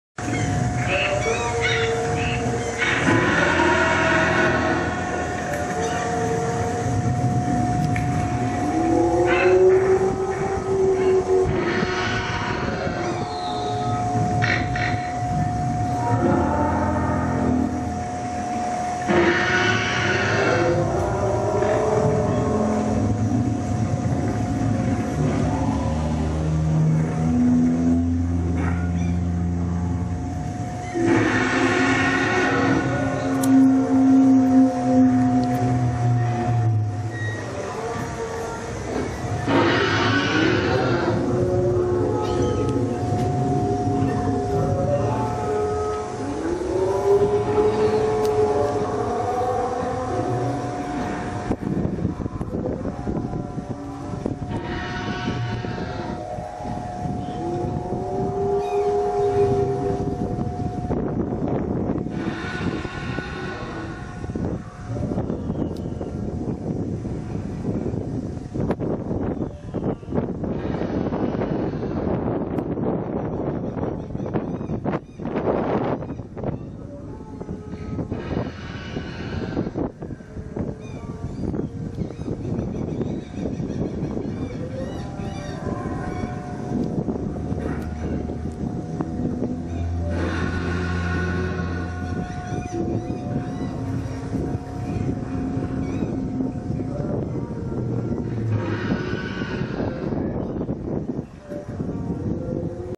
Dinosaures à Sucre, Bolivie
Parc dinosaures à Sucre, Bolivie. Traces de dinosaures miraculeusement conservées dans le sol.